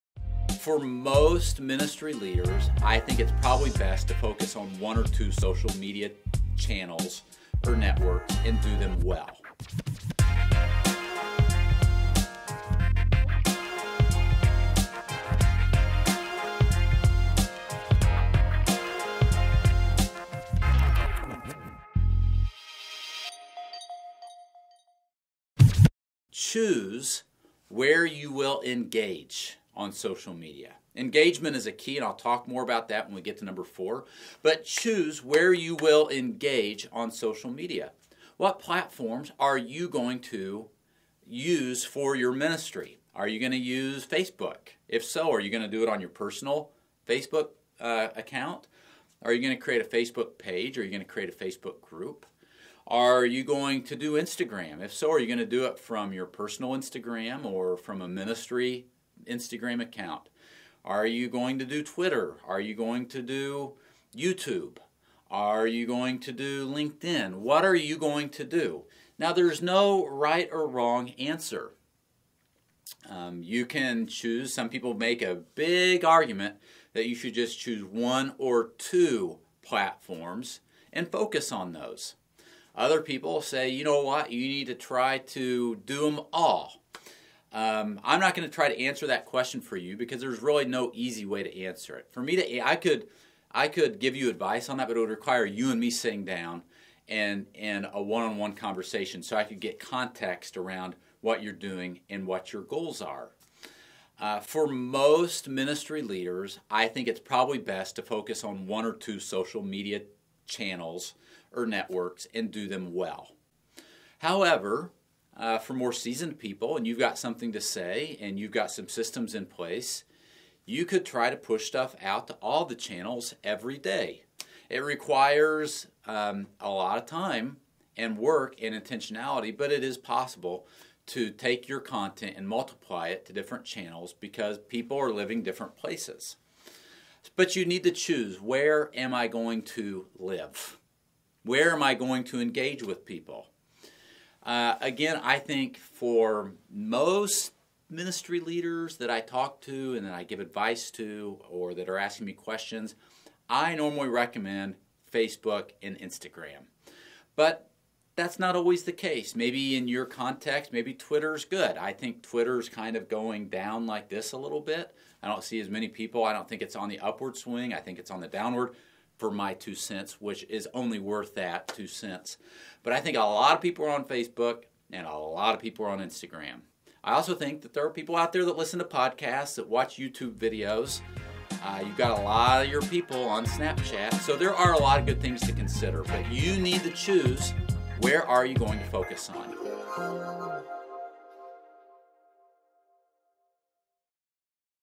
Social Media Strategy Tip #2 Choose Where You Will Engage
With so many options available, what is the best social media platform for you? That's not an easy question to answer. If you're struggling with this question, take a few minutes and find out what Ryan Frank has to say.